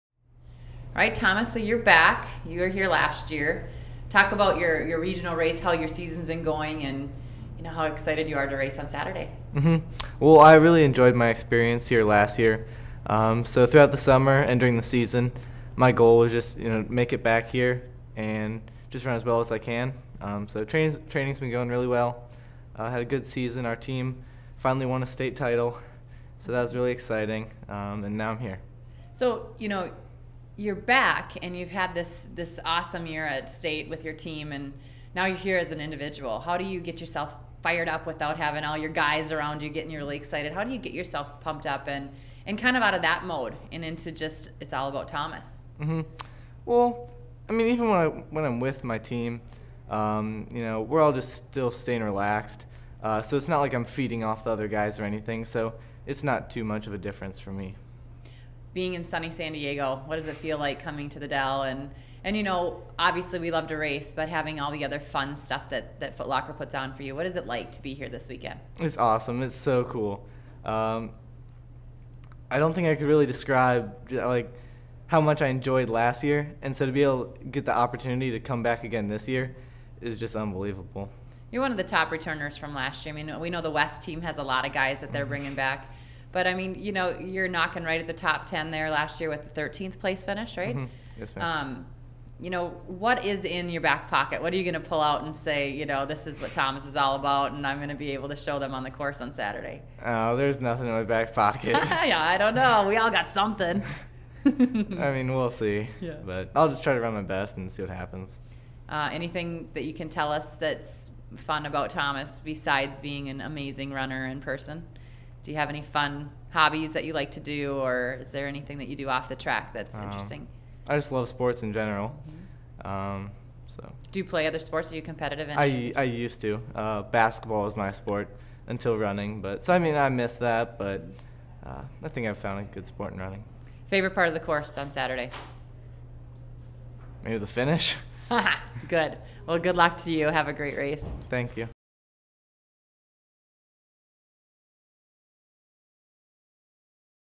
Thomas Pollard
Thomas Pollard qualified for the 2014 Foot Locker Cross Country Championships